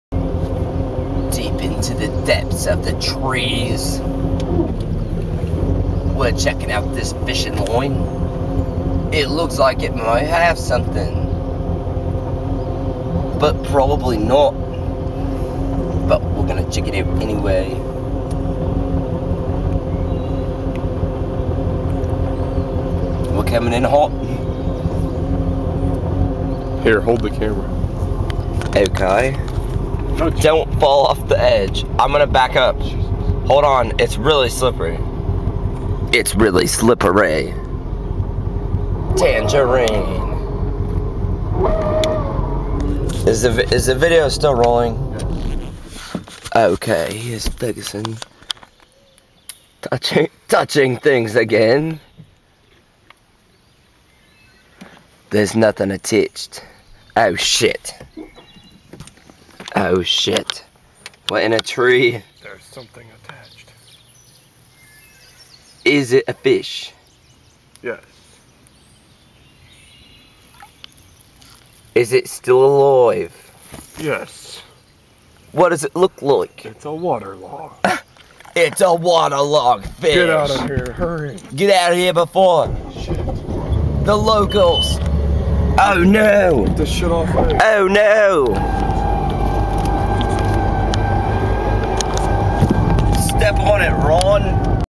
Funny Safari Video Water Edition 3
Ron and Ferguson find a fishing line